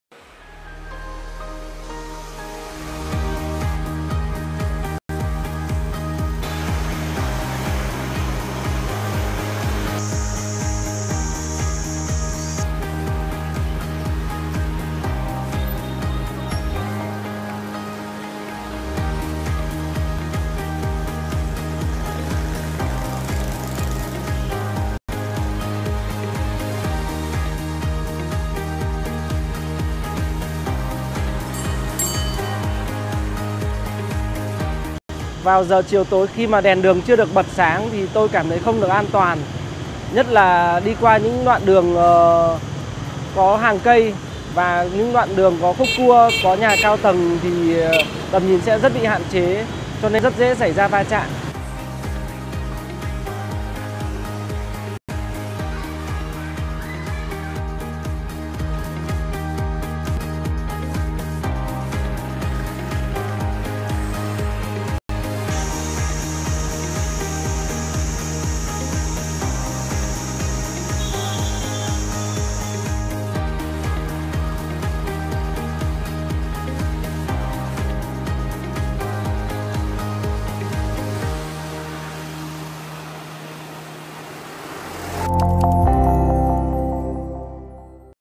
Đường phố, công viên Hà Nội thiếu ánh sáng vì cắt giảm điện
Nguồn Zingnews.vn